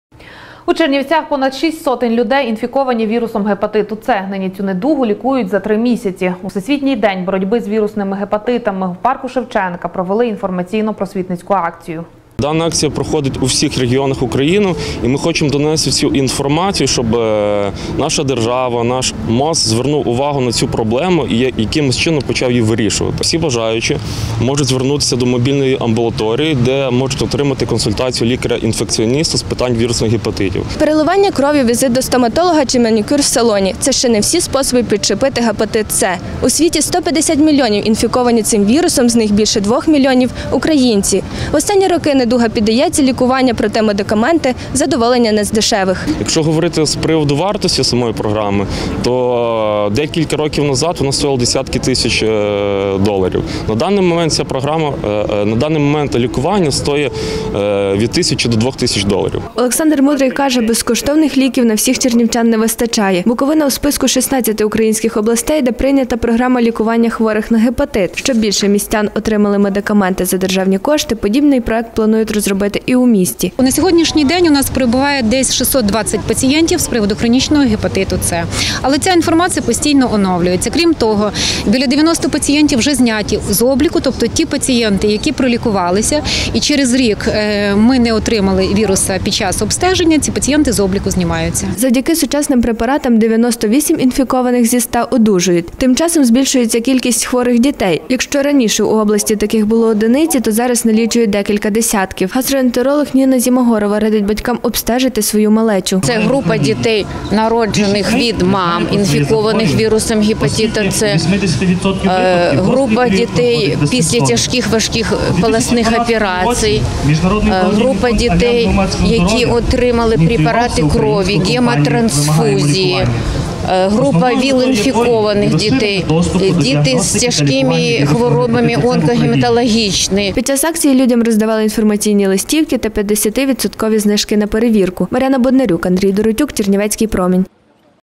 Понад шість сотень чернівчан інфіковані вірусом гепатиту С
Хвороба цілком виліковна, але для цього необхідно пройти тримісячний курс. У Всесвітній день боротьби з вірусними гепатитами у парку Тараса Шевченка чернівецькі медики провели інформаційно-просвітницьку акцію.